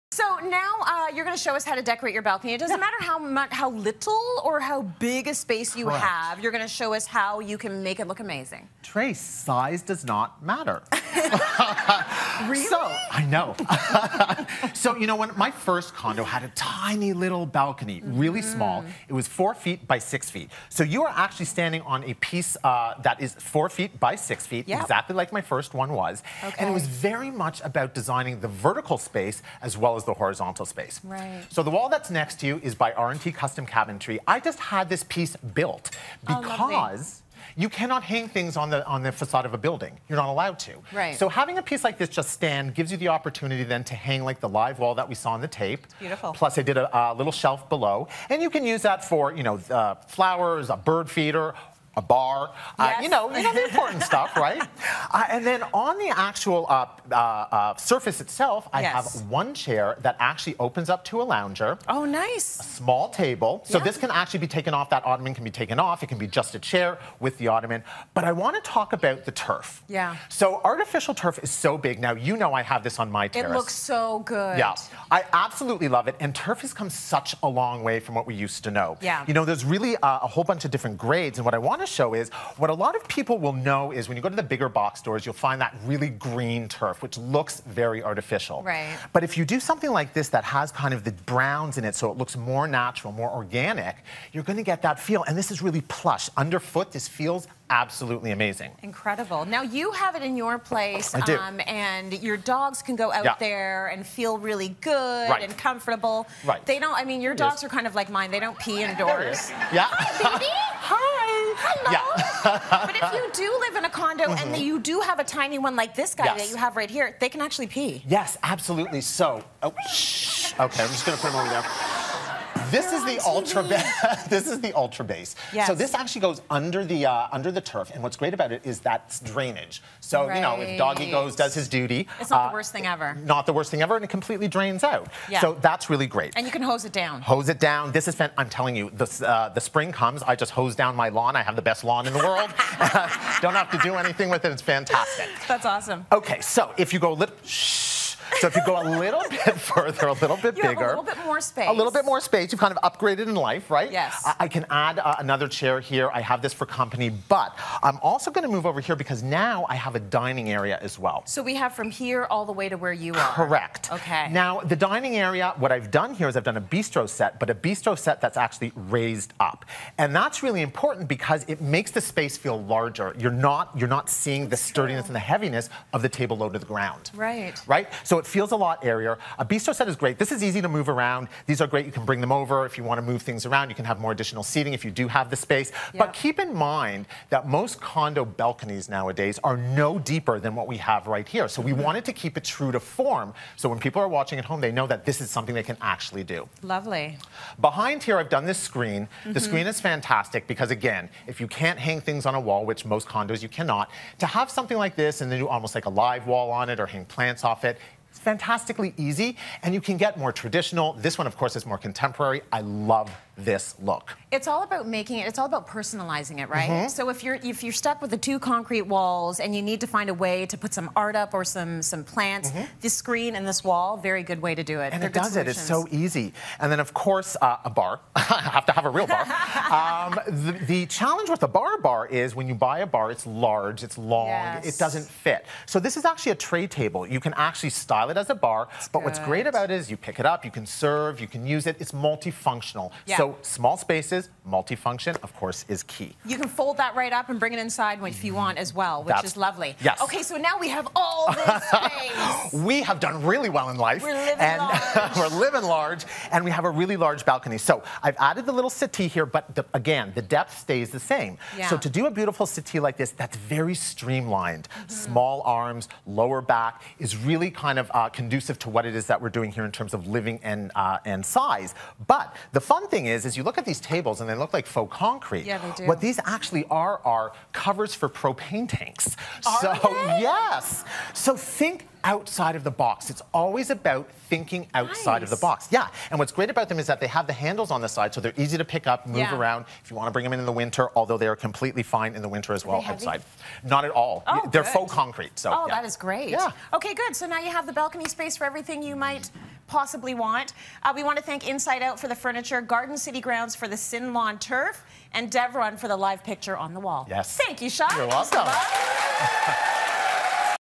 The balcony design techniques that prove size doesn't matter
Make your balcony POP with Designer Shai DeLuca's tips and tricks.